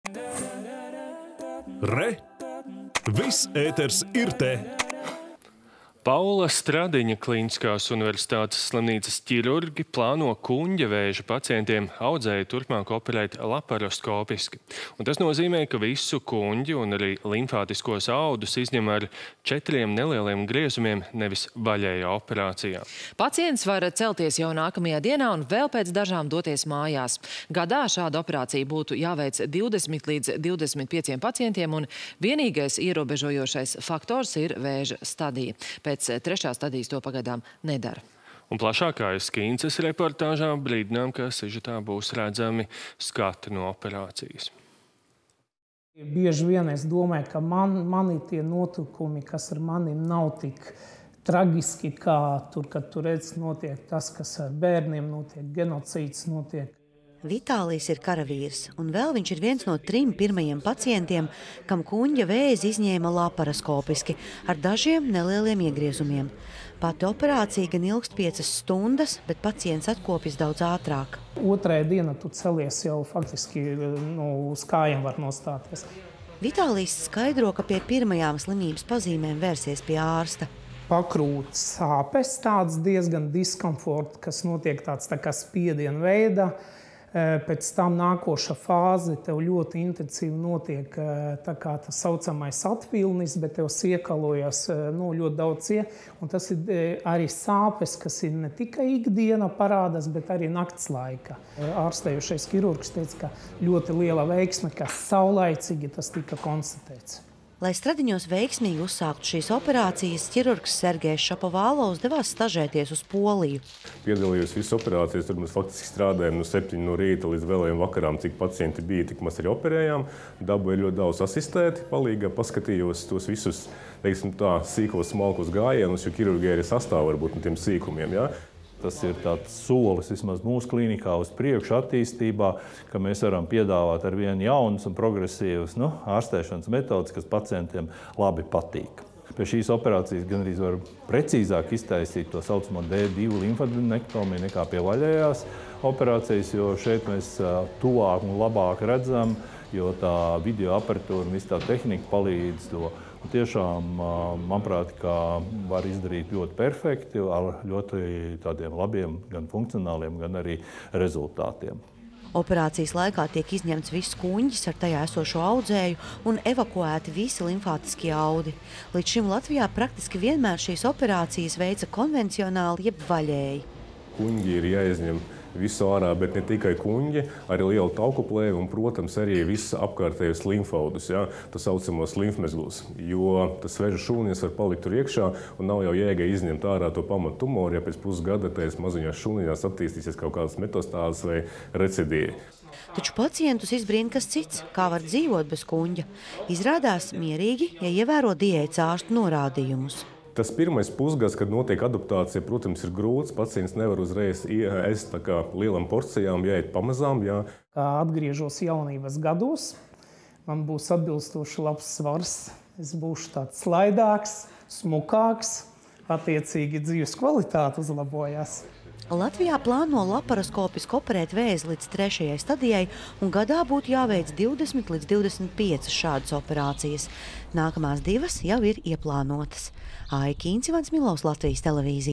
Kuņģa vēzi Stradiņos operē maksimāli saudzējoši
Paula Stradiņa klīniskās universitātes slimnīcas ķirurgi plāno kuņģa vēža pacientiem audzēju turpmāk operēt laparoskopiski. Tas nozīmē, ka visu kuņģi un arī limfātiskos audus izņem ar četriem nelieliem griezumiem, nevis vaļējā operācijā. Pacients var celties jau nākamajā dienā un vēl pēc dažām – doties mājās. Gadā šāda operācija būtu jāveic 20 līdz 25 pacientiem, un vienīgais ierobežojošais faktors ir vēža stadija. Pēc trešās stadijas to pagaidām nedara.